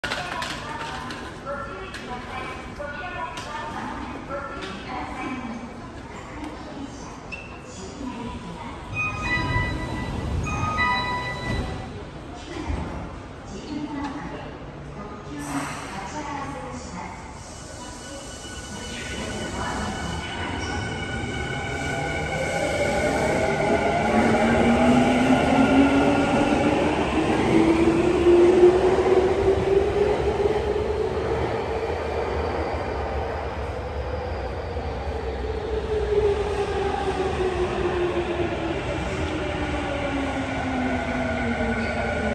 【まもなくリニューアル】東急5000系5121F SDGsトレイン 美しい時代へ号 各駅停車 元町・中華街行き みなとみらい駅発車
現在東横線では、ＳＤＧｓトレイン 美しい時代へ号が運行されています。数日前にリニューアルが発表され、2023年4月から新たなデザインへと変更される予定です。まもなく見納めとなるこのラッピングを、みなとみらい駅にて撮影しました。